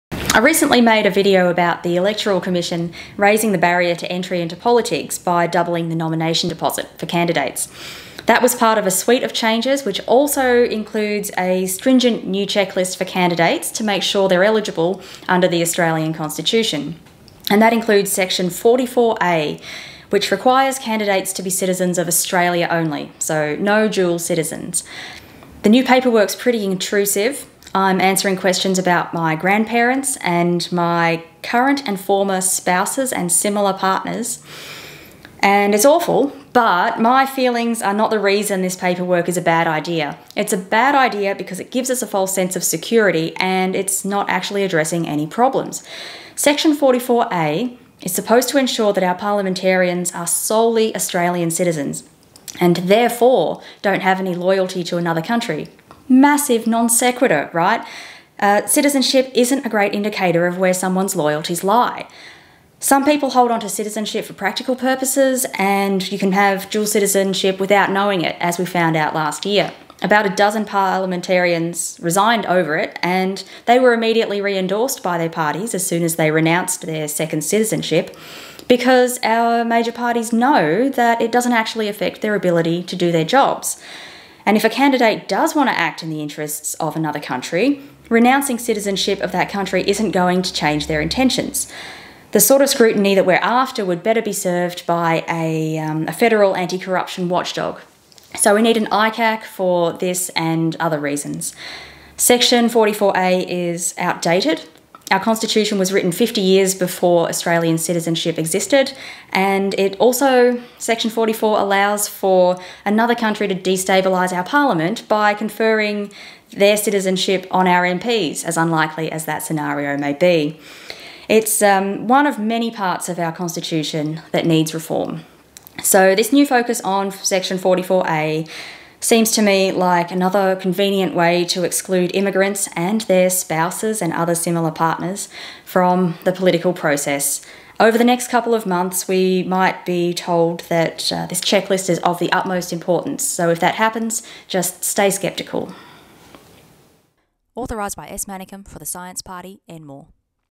New Australians Need Not Apply
Dual citizens of Australia can't be nominated for federal parliament, and that's just one part of Section 44 of our Constitution. Holding -- or renouncing -- dual citizenship doesn't necessarily indicate a person's loyalties.

NOTE: Andrea says section "44a" in the video, but it should be "44i".